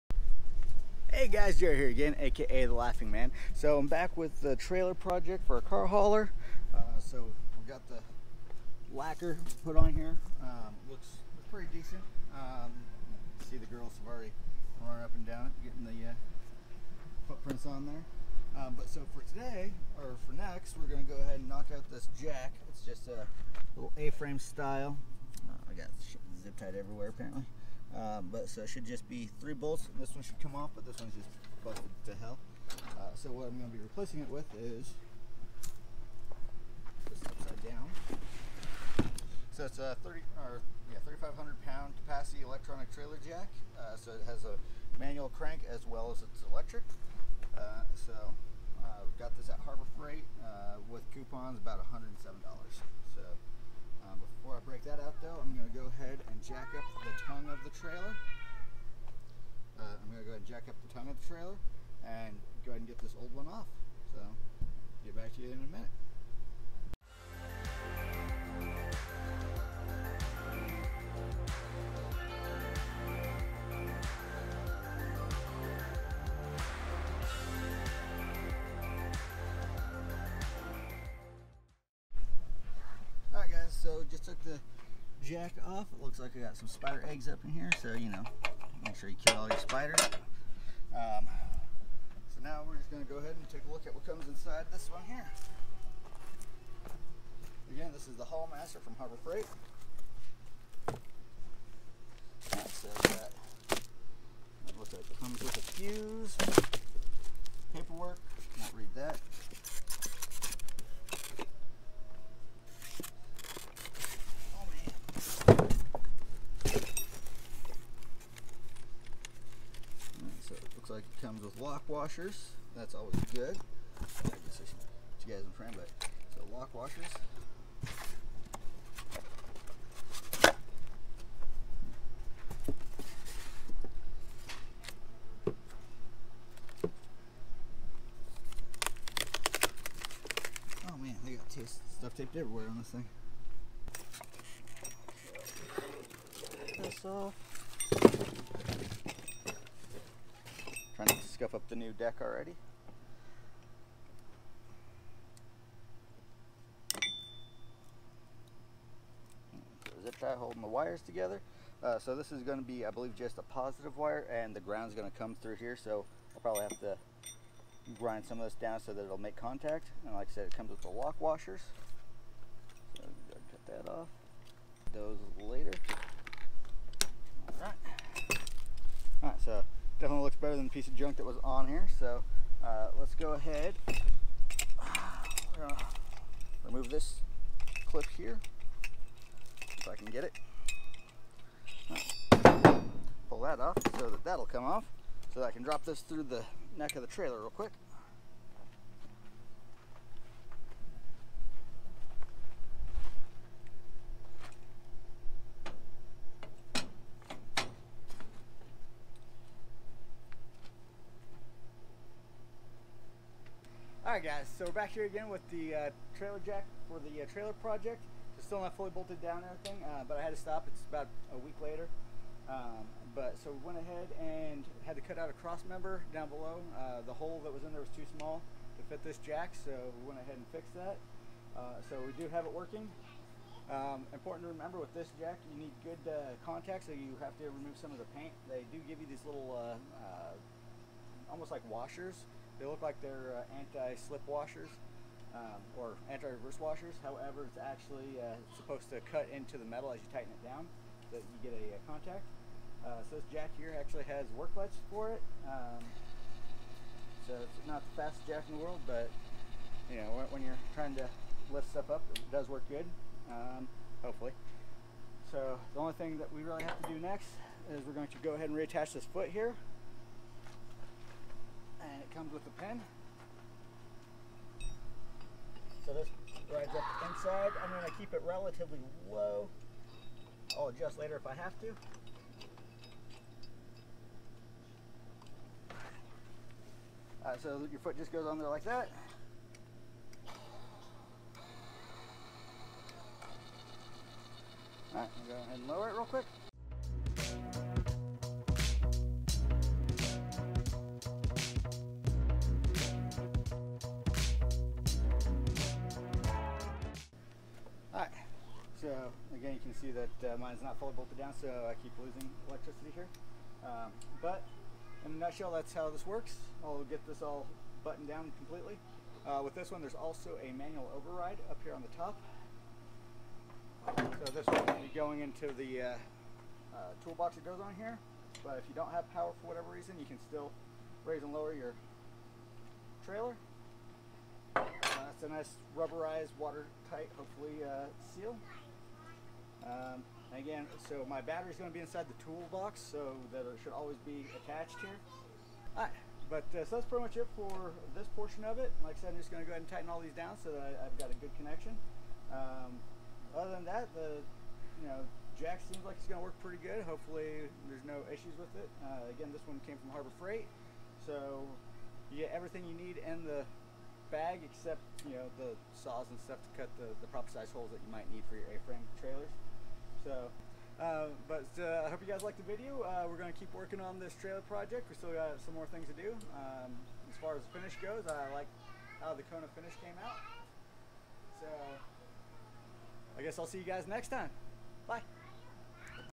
Car hauler project part 2 Harbor freight Jack
The second part of my new car hauler build.

This video was filmed on my:
GoPro Hero 7
with the Rode video micro.

Products Used in this video:
HAUL-MASTER
3500 Lb. A-Frame Heavy Duty Electric Trailer Jack With Drop Leg